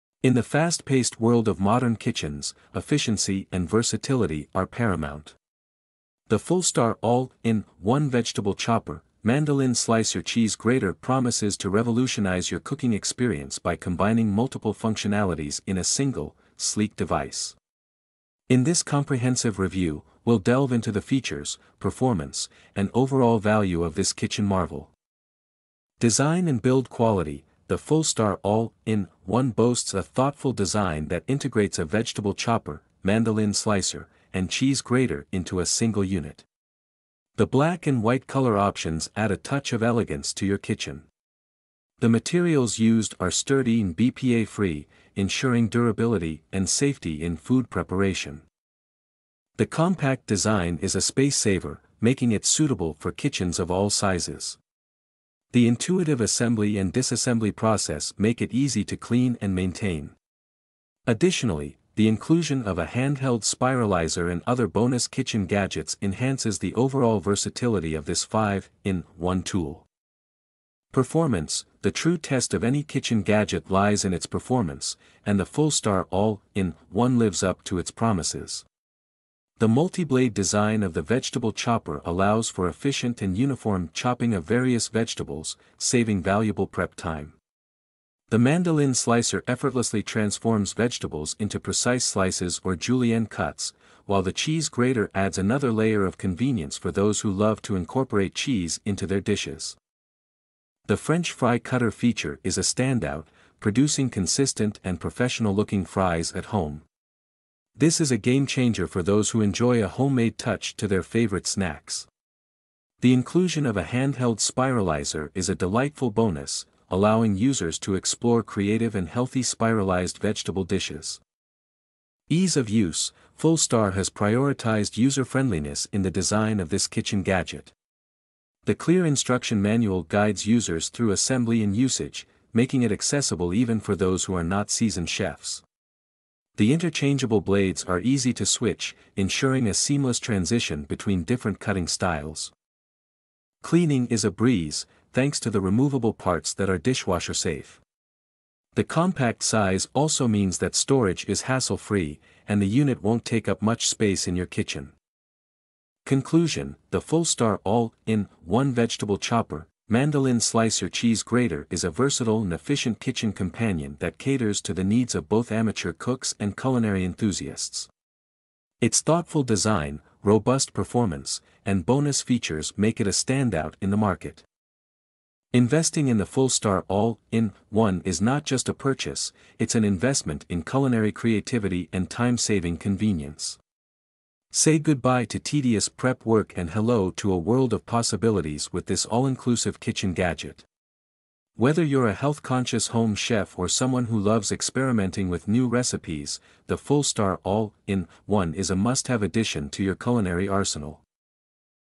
Unleashing Culinary Creativity with Fullstar's All in 1 Vegetable Chopper A Comprehensive Review
Introduction:
In the fast-paced world of modern kitchens, efficiency and versatility are paramount. The Fullstar All-in-1 Vegetable Chopper, Mandoline Slicer & Cheese Grater promises to revolutionize your cooking experience by combining multiple functionalities in a single, sleek device. In this comprehensive review, we'll delve into the features, performance, and overall value of this kitchen marvel.

Design and Build Quality:
The Fullstar All-in-1 boasts a thoughtful design that integrates a vegetable chopper, mandoline slicer, and cheese grater into a single unit. The black and white color options add a touch of elegance to your kitchen. The materials used are sturdy and BPA-free, ensuring durability and safety in food preparation.

The compact design is a space-saver, making it suitable for kitchens of all sizes. The intuitive assembly and disassembly process make it easy to clean and maintain. Additionally, the inclusion of a handheld spiralizer and other bonus kitchen gadgets enhances the overall versatility of this 5-in-1 tool.

Performance:
The true test of any kitchen gadget lies in its performance, and the Fullstar All-in-1 lives up to its promises. The multi-blade design of the vegetable chopper allows for efficient and uniform chopping of various vegetables, saving valuable prep time. The mandoline slicer effortlessly transforms vegetables into precise slices or julienne cuts, while the cheese grater adds another layer of convenience for those who love to incorporate cheese into their dishes.

The French fry cutter feature is a standout, producing consistent and professional-looking fries at home. This is a game-changer for those who enjoy a homemade touch to their favorite snacks. The inclusion of a handheld spiralizer is a delightful bonus, allowing users to explore creative and healthy spiralized vegetable dishes.

Ease of Use:
Fullstar has prioritized user-friendliness in the design of this kitchen gadget. The clear instruction manual guides users through assembly and usage, making it accessible even for those who are not seasoned chefs. The interchangeable blades are easy to switch, ensuring a seamless transition between different cutting styles.

Cleaning is a breeze, thanks to the removable parts that are dishwasher-safe. The compact size also means that storage is hassle-free, and the unit won't take up much space in your kitchen.

Conclusion:
The Fullstar All-in-1 Vegetable Chopper, Mandoline Slicer & Cheese Grater is a versatile and efficient kitchen companion that caters to the needs of both amateur cooks and culinary enthusiasts. Its thoughtful design, robust performance, and bonus features make it a standout in the market.

Investing in the Fullstar All-in-1 is not just a purchase; it's an investment in culinary creativity and time-saving convenience. Say goodbye to tedious prep work and hello to a world of possibilities with this all-inclusive kitchen gadget. Whether you're a health-conscious home chef or someone who loves experimenting with new recipes, the Fullstar All-in-1 is a must-have addition to your culinary arsenal.

Fullstar All-in-1 Vegetable Chopper